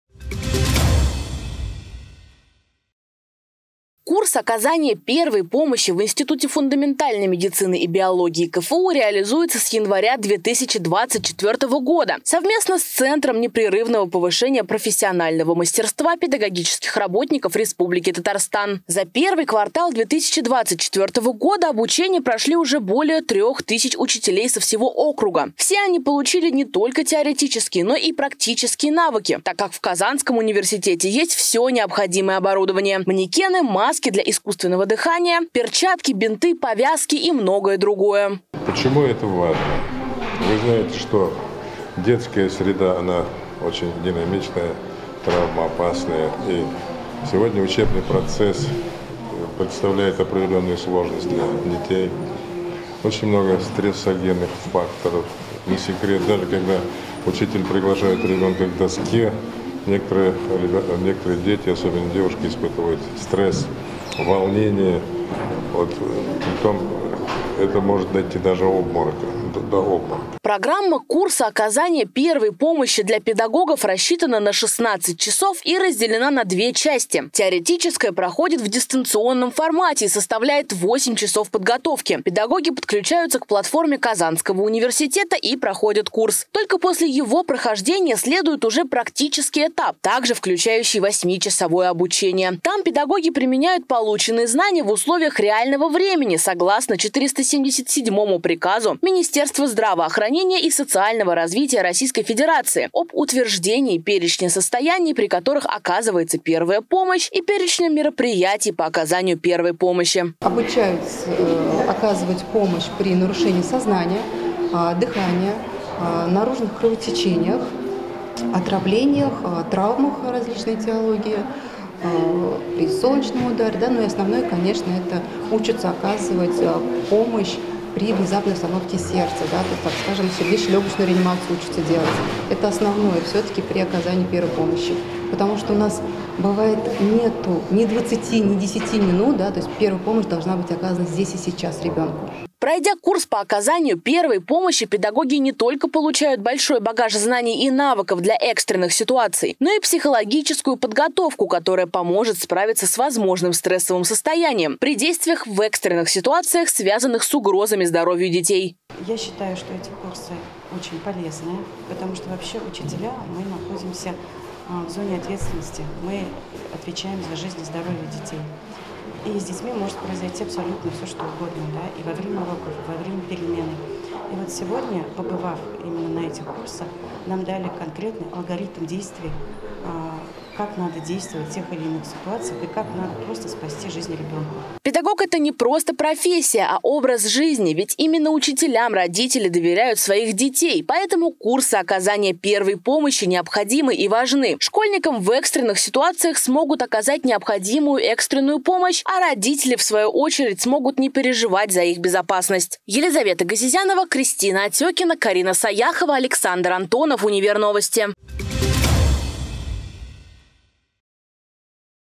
КАК УЧИТЕЛЬ МОЖЕТ СПАСТИ ЖИЗНЬ ШКОЛЬНИКУ?
Школьная жизнь полна эмоций и событий: первая линейка, интересный урок по астрономии, постановка танцевальных номеров, изготовление скворечников, весёлые переменки, обретение крепкой дружбы или даже неожиданной  влюблённости. Но в учебных заведениях часто возникают нештатные ситуации: от незначительных травм до неожиданных серьёзных происшествий. Оказание первой помощи ложится на учителей, поэтому они должны обладать определёнными навыками, чтобы быстро и правильно реагировать на такие ситуации. В Казанском федеральном университете как раз реализуют модуль курса оказания первой помощи для педагогов школ Республики Татарстана. Подробнее о программе узнаете в следующем сюжете.